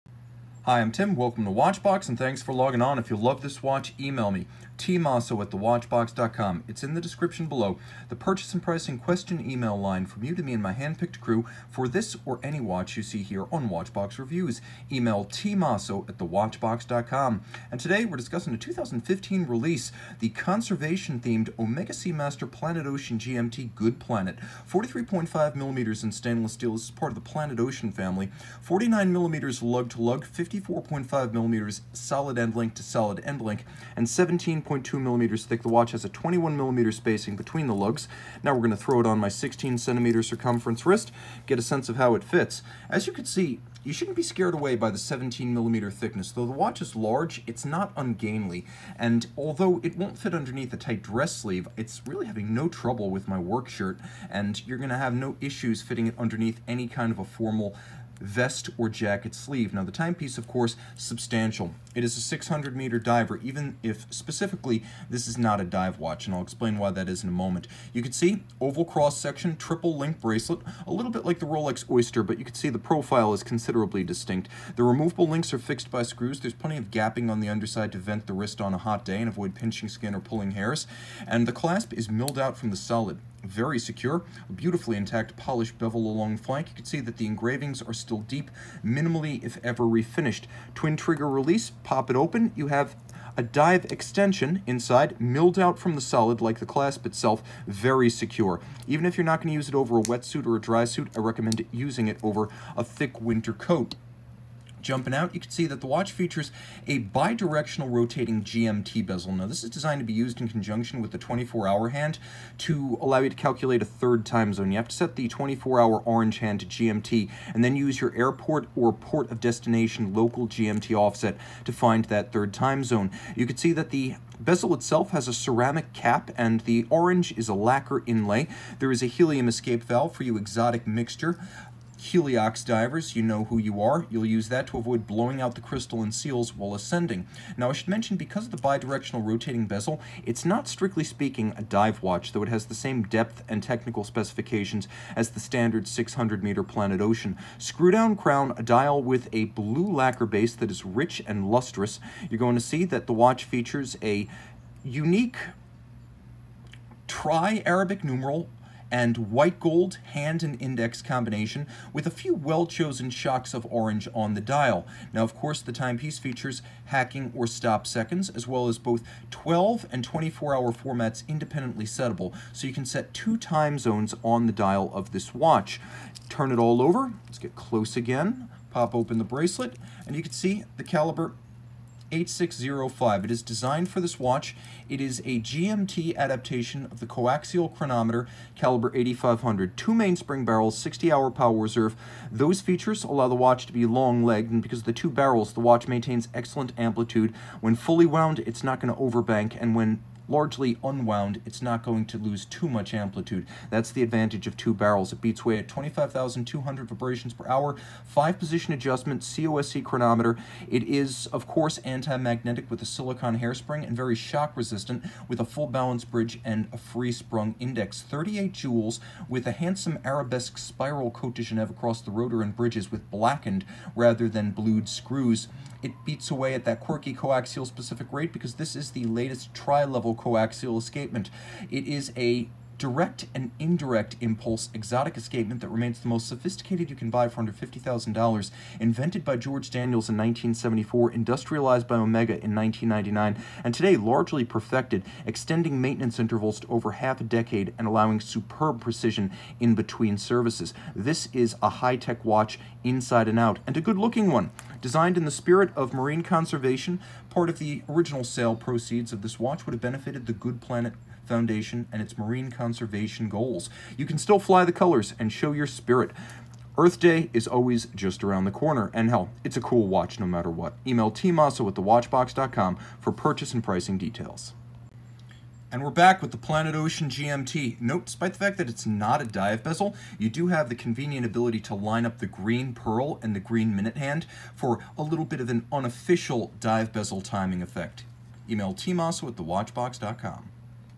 Omega Seamaster Planet Ocean 600M GMT "GoodPlanet" 232.30.44.22.03.001 Omega Watch Review
The Omega Seamaster Planet Ocean 600M GMT "GoodPlanet" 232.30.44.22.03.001 is encased in 43.5mm of stainless steel surrounding a blue dial on a stainless steel bracelet. Features of this Omega Seamaster Planet Ocean include bidirectional GMT bezel, hours, minutes, seconds, date, GMT, 600-meter water resistance, helium escape valve, COSC Swiss chronometer certification, and diving clasp with dive extension.

For complete details, watch the full Omega Seamaster Planet Ocean review by Tim Mosso!